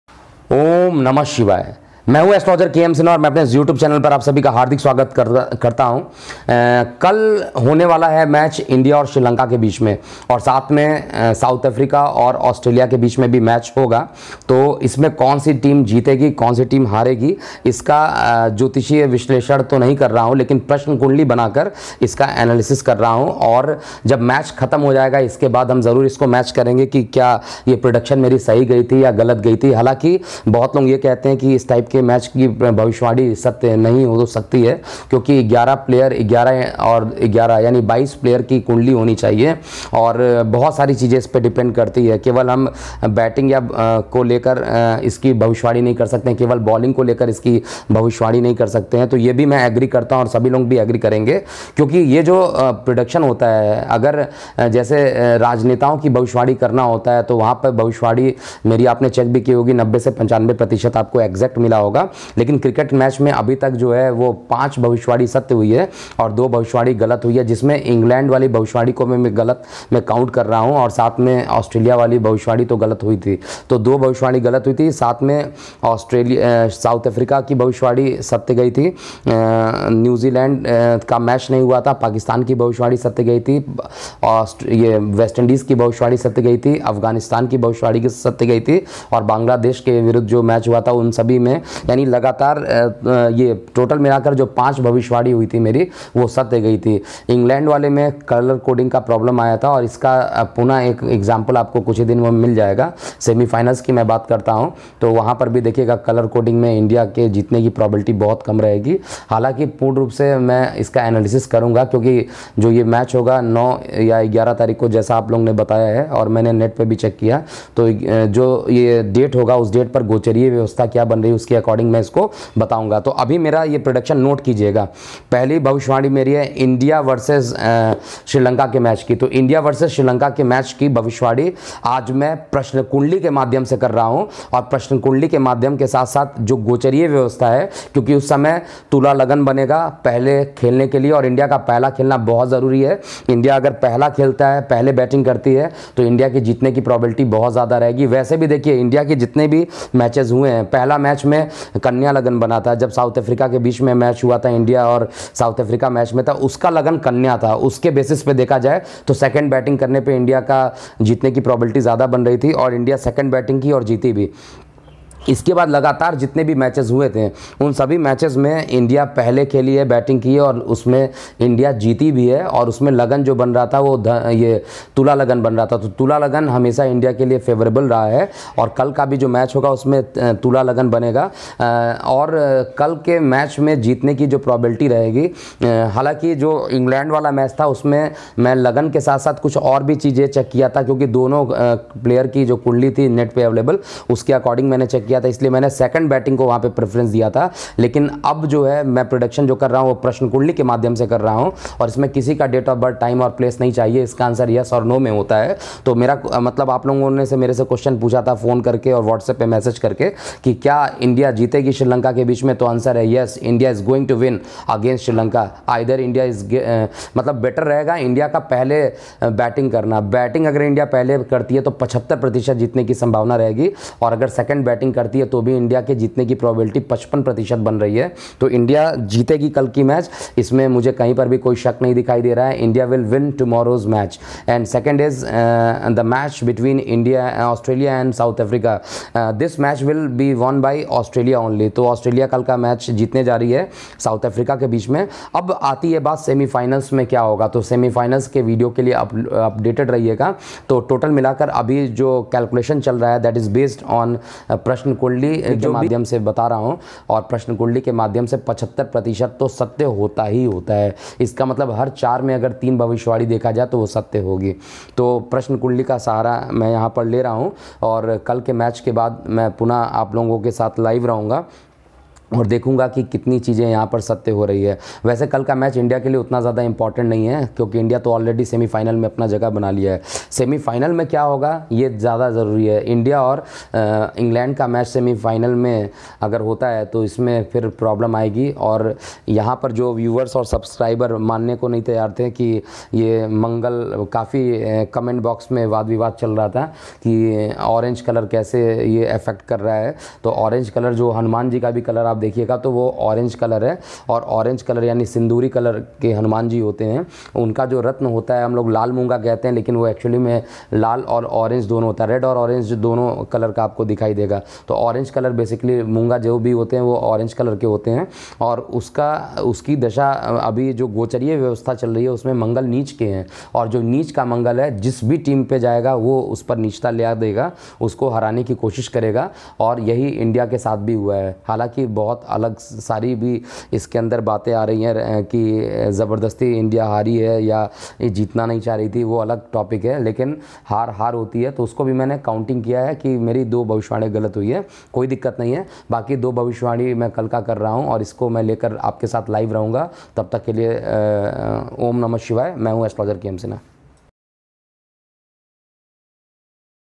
India_Vs_ Srilanka & Australia_Vs_South_Africa By Astrologer KM SINHA
Dear Viewer's and subscriber Please note in india vs Srilanka winning probability of india is 75% if batting first and 55% if batting second.
For Australia and South Africa Winning probability of Australia is 75% is batting first and otherwise only 50% -50% . Any team can win if Australia batting second